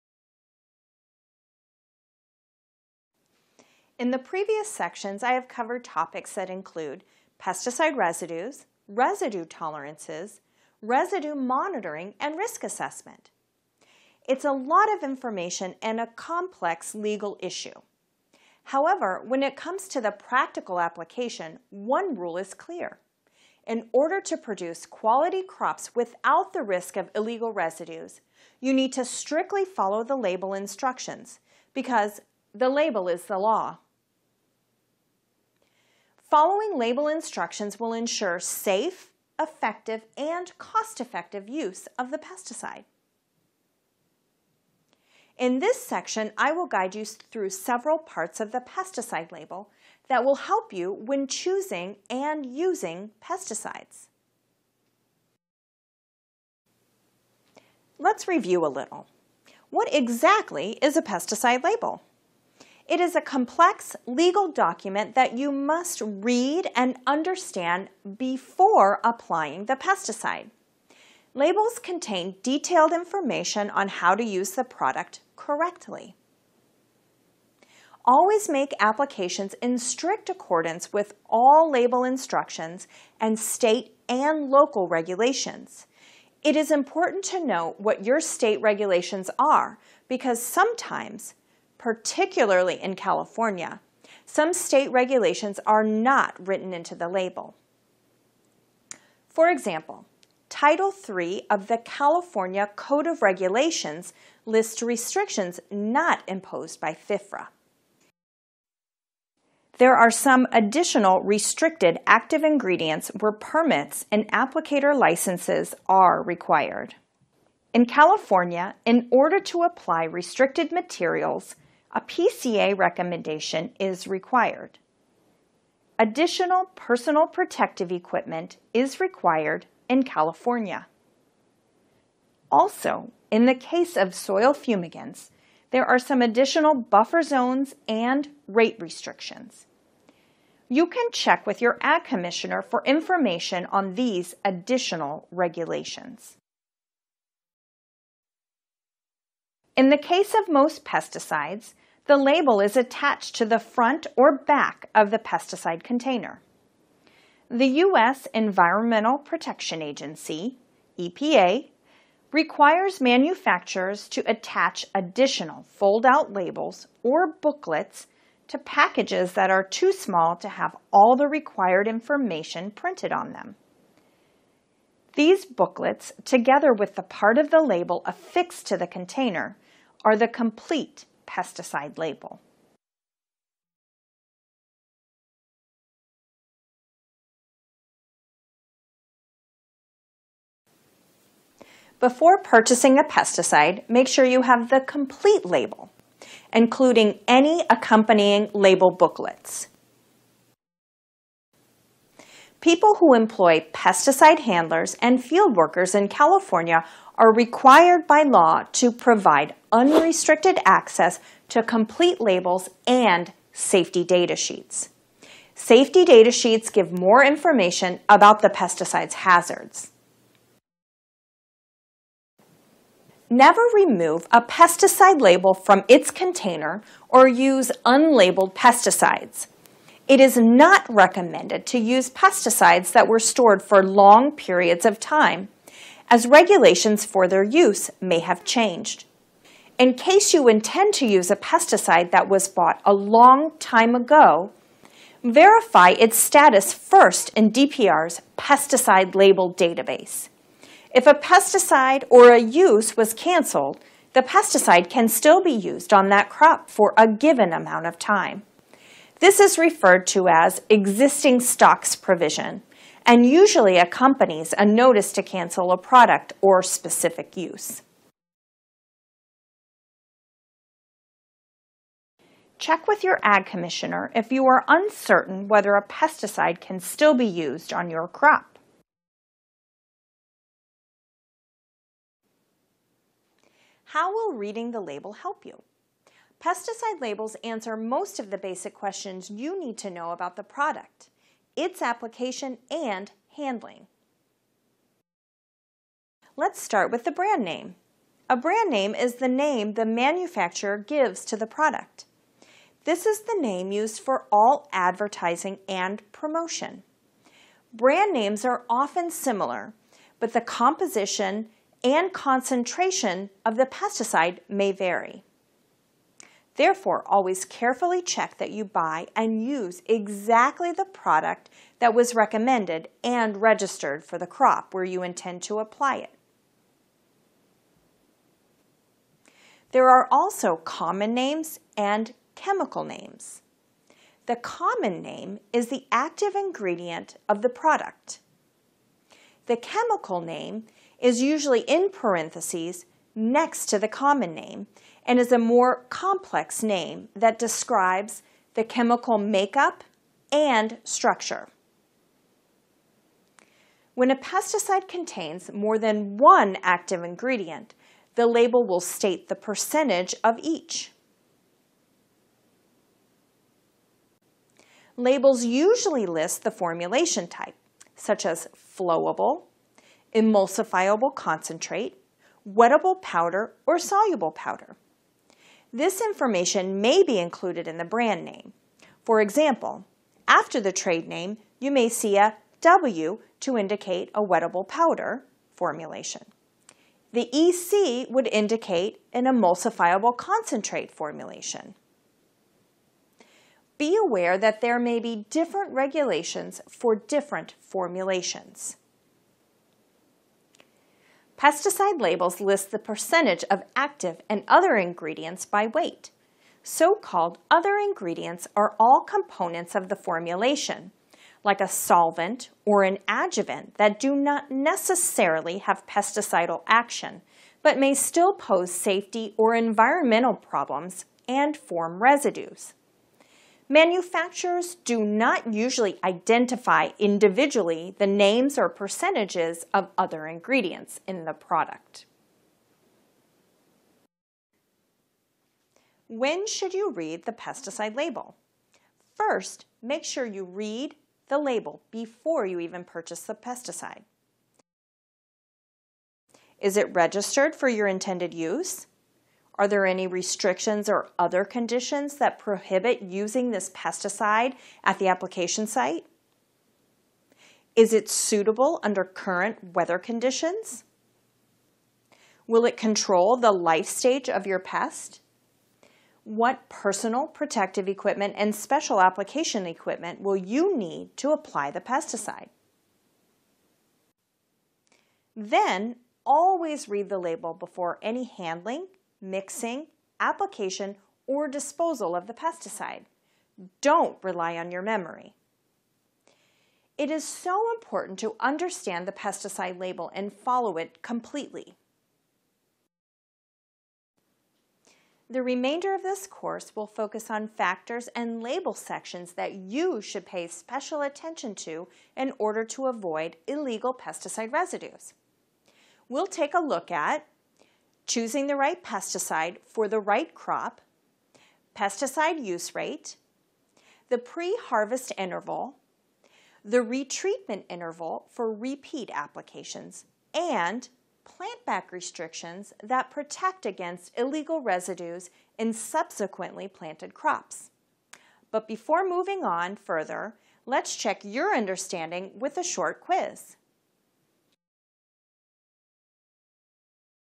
Proper Pesticide Use to Avoid Illegal Residues: Section 5 - The Pesticide Label
This video is part of a series. To see all videos in the playlist, Proper Pesticide Use to Avoid Illegal Residues course provides information on pesticide residues, monitoring, tolerances, and risk assessment, and focuses on important sections of the pesticide label, presenting real-life scenarios where the most important factors leading to illegal residues are identified. To take this course for CE units and to find out more about UC IPM online courses